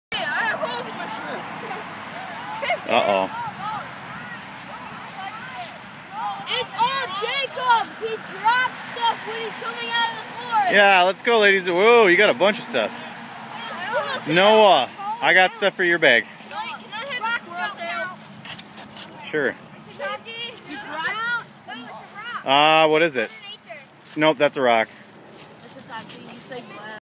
Sioux Trail Cleanup 8 (2012)
Fifth Graders do their part by cleaning up Sioux Trail's surrounding areas!